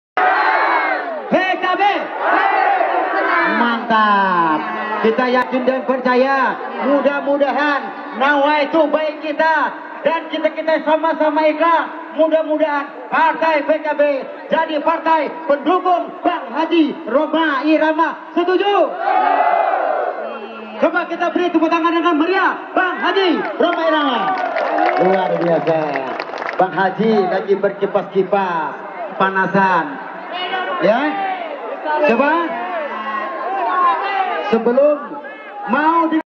Duet MC Baju Batik Kampanye Nasional PKB Bengkulu 2014
Haji Rhoma Irama dan Soneta Group menggebrak lapangan Padang Jaya, Bengkulu Utara, dalam acara spektakuler Kampanye Nasional PKB 2014. Acara bertajuk Konser Rakyat PKB itu dihadiri ribuan massa dari segala penjuru Bengkulu dan dihadiri Jurkamnas KH Wahid Nuruddin dan Jurkamda KH Ahmad Daraini. Ketua Panitia Kampanye di Bengkulu, Jhon Ramadhan, mengatakan kehadiran Rhoma Irama di Bengkulu sebagai bukti cinta Partai Kebangkitan Bangsa (PKB) kepada rakyat Bengkulu. -video: @kholilpayaman